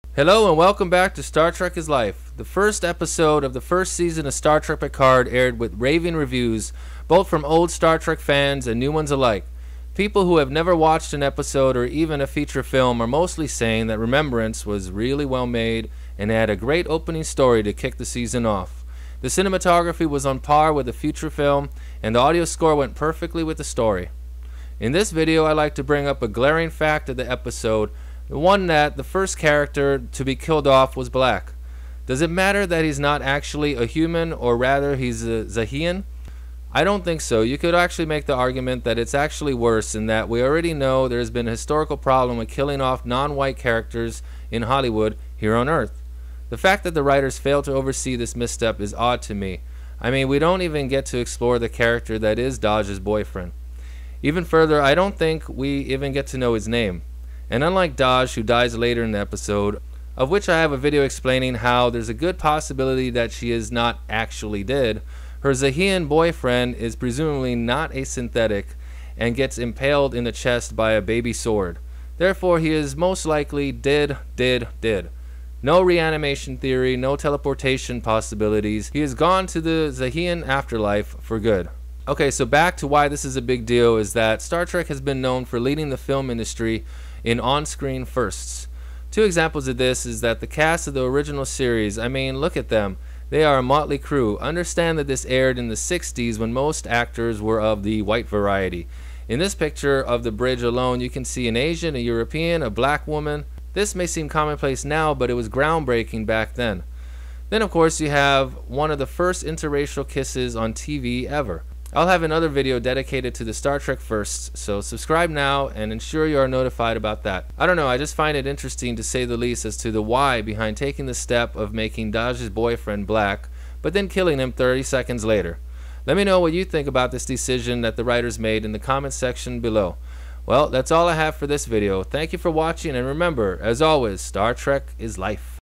Star Trek Picard First Character Killed Off Is Black
Why in the world is Star Trek, a franchise known to be inclusive and forward thinking, decide the first character to die would be black?